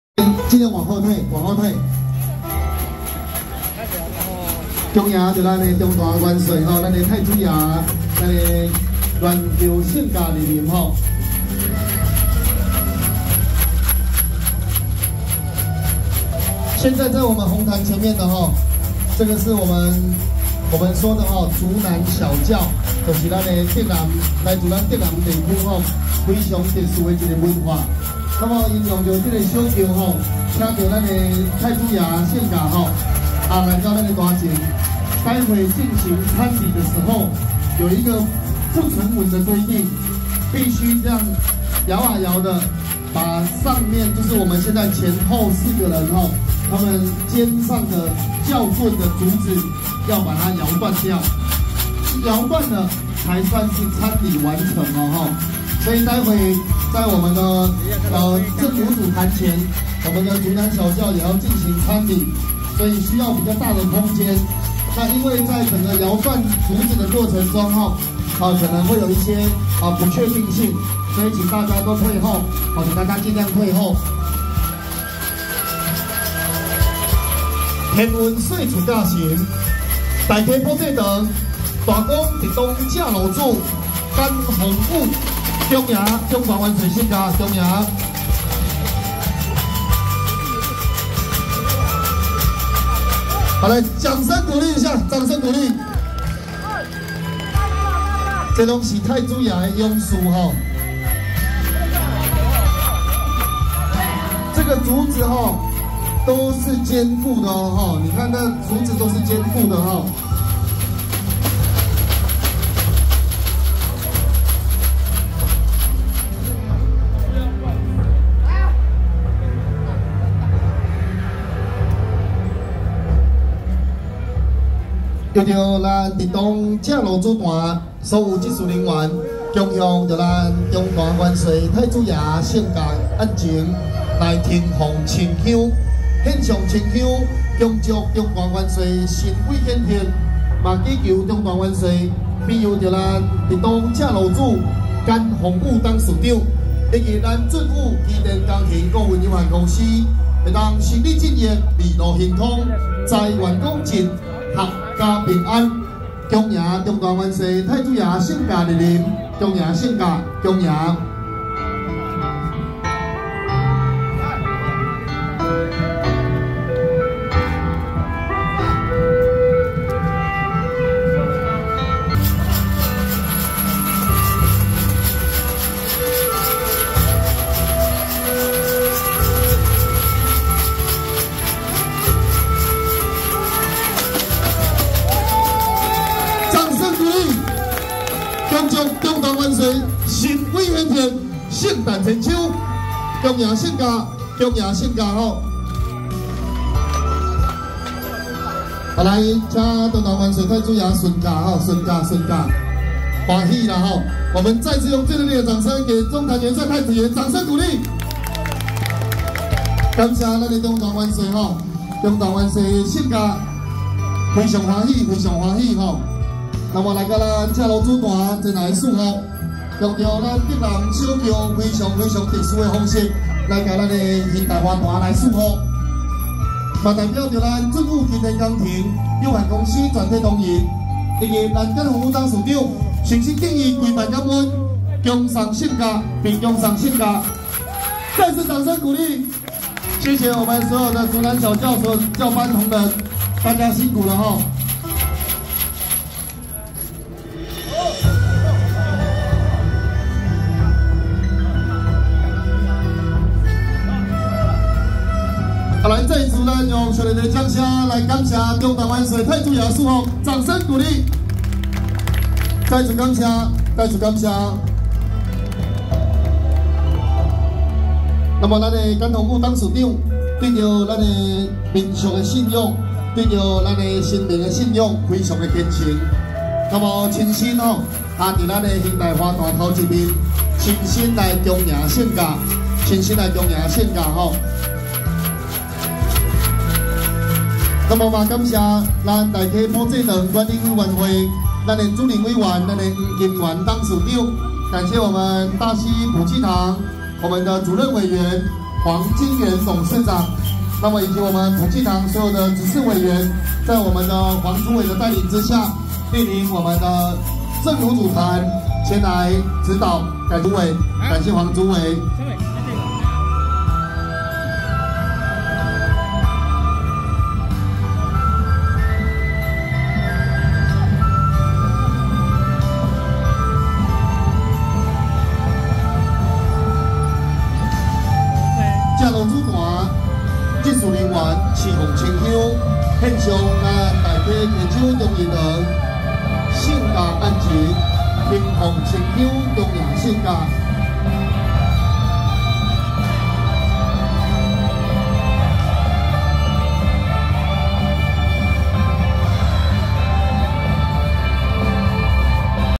【竹南小轎】~113大溪普濟堂恭祝關聖帝君聖誕遶境
竹南小轎